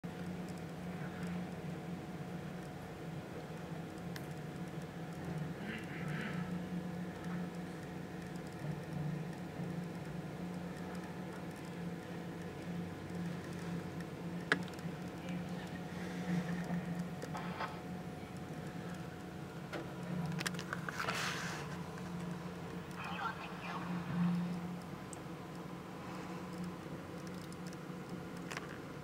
Tearing Down the Whitney Building
The northern portion of the Whitney Building at Sheridan College has been torn down to make way for a significant expansion of the art and music wing.